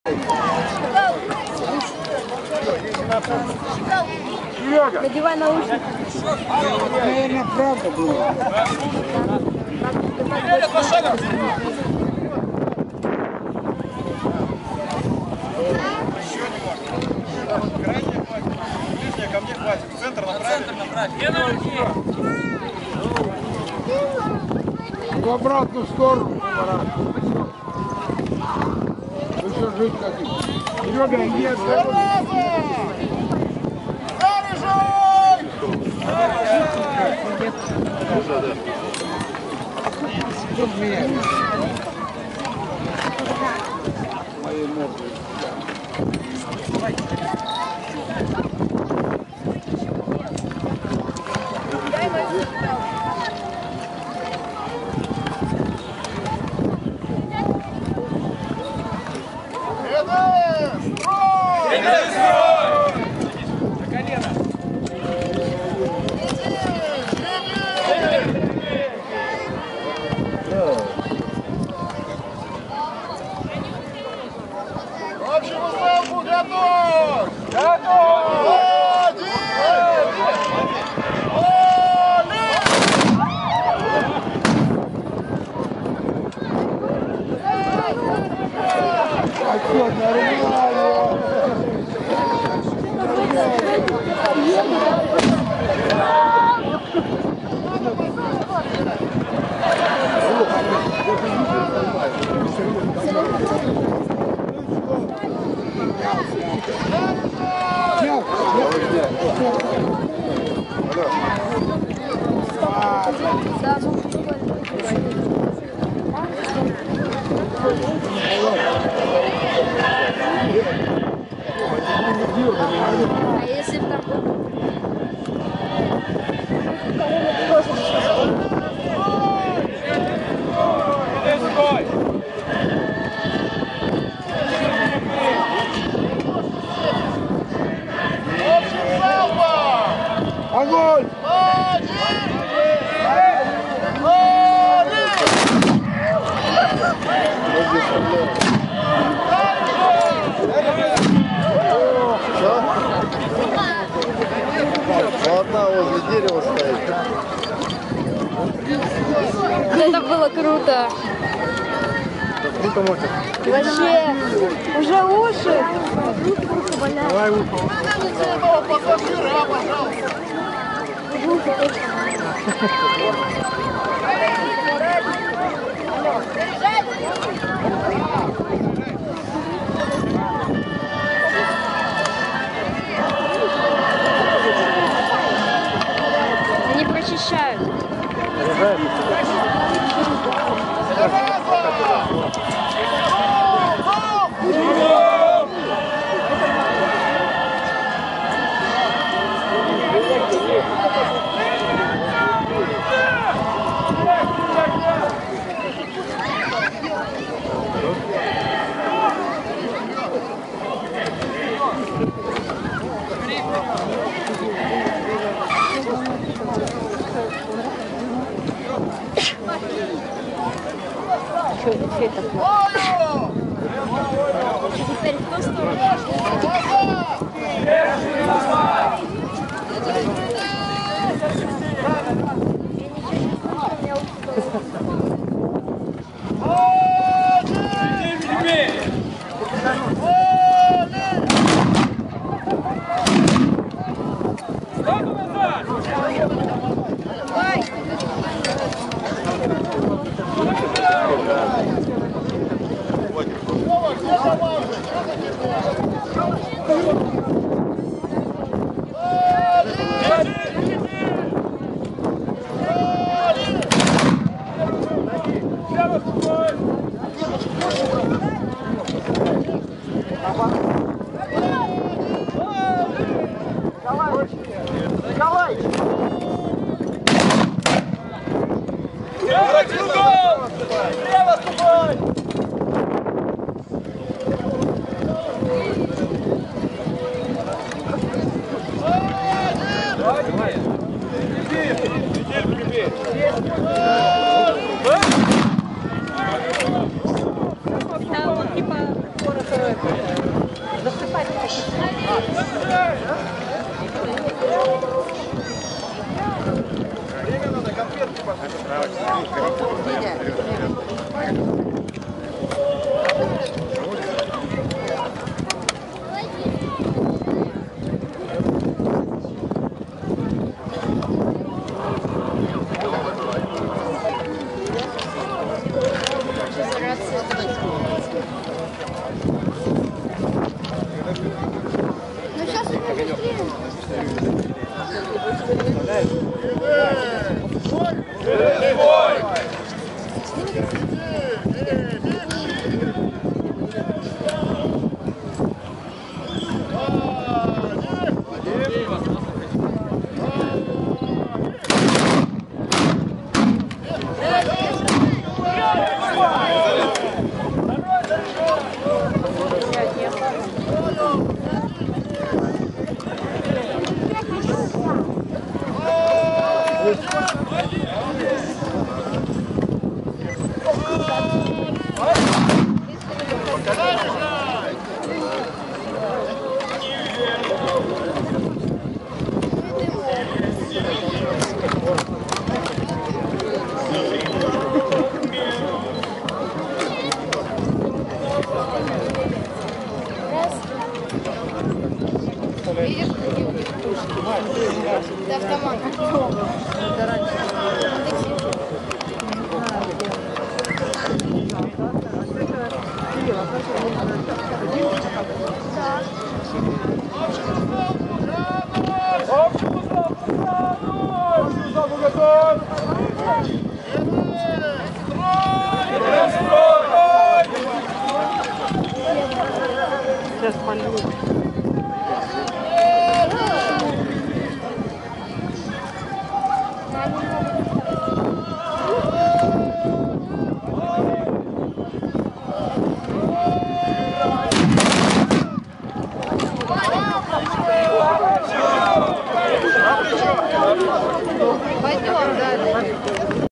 Фестиваль реконструкторов Сибирский огонь 2014 (Новосибирск), Часть 3
Все части фоторепортажа и видеоматериалов с фестиваля Сибирский Огонь 2014 на страницах Новосибирского Фото Блога: